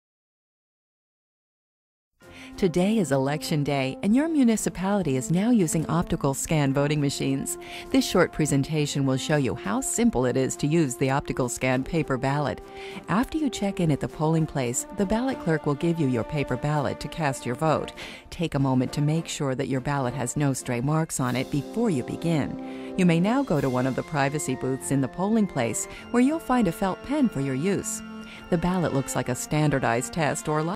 ORANGE, CT Optical Scan Voting Machine OGAT EVENT
A short film on using the new optical scan voting machines to be used to vote in all upcoming elections in the state of Connecticut.

OGAT